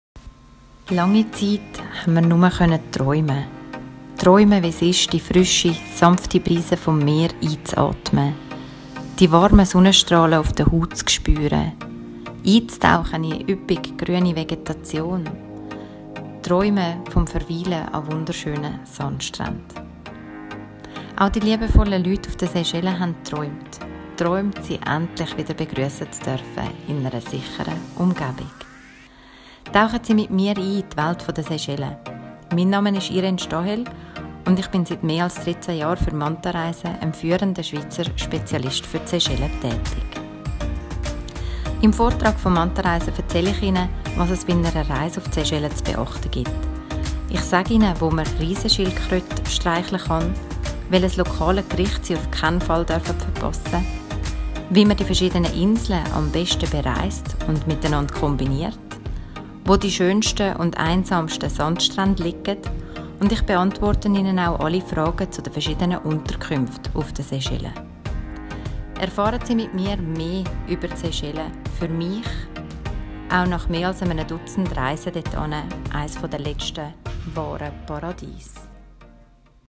Seychellen – Trauminsel entdecken und erleben
Eines der letzten wahren Paradiese – die Seychellen. Steil aus dem Meer herausragende Granitfelsen, herrliche Palmenstrände, kristallklares Wasser, üppige Vegetation, kühle Regenwälder und eine beeindruckende Flora und Fauna. Entdecken und erleben Sie mit mir die faszinierende Welt der Seychellen. Erfahren Sie mehr über die nachhaltigste Destination im Indischen Ozean, wie Sie die Inseln am besten bereisen und welche Unterkünfte wir Ihnen persönlich empfehlen.